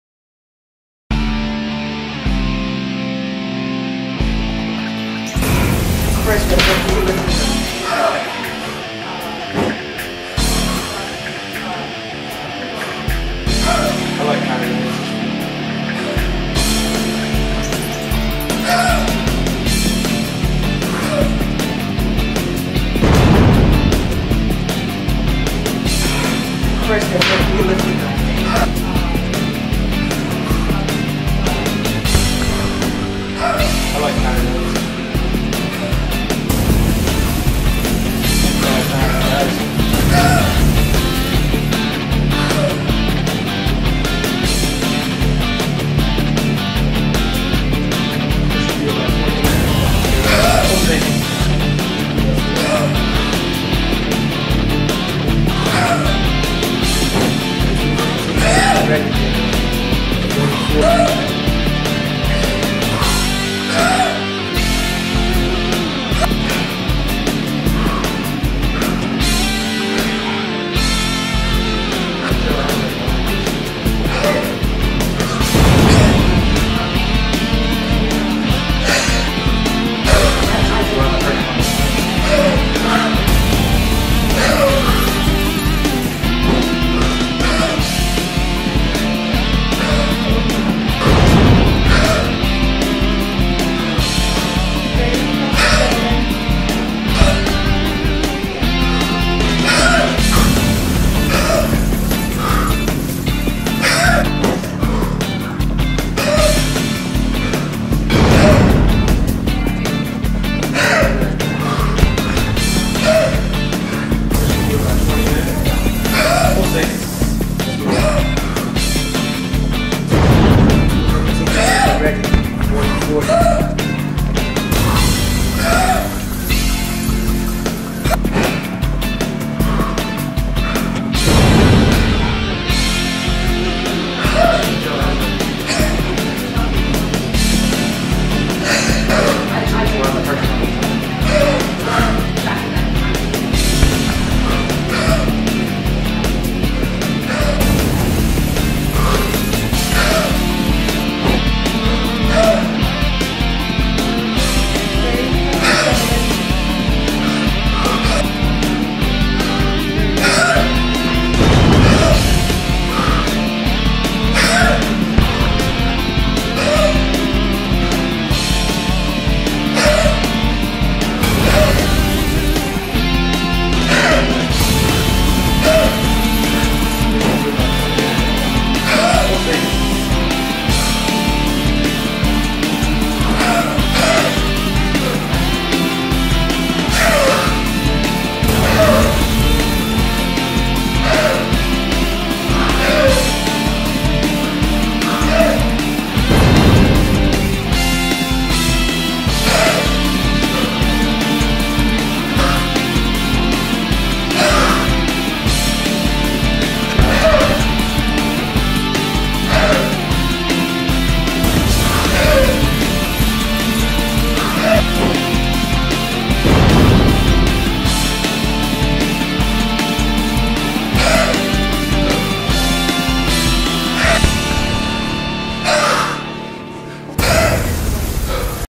Coaches Chris D and Joe C / Team KBNY
Just another workout with the 32kg / 70lb kettlebell. The boys exhibit high level technique, while working with a relatively heavy weight, at a rapid pace... it just doesn't get any better than that. for more.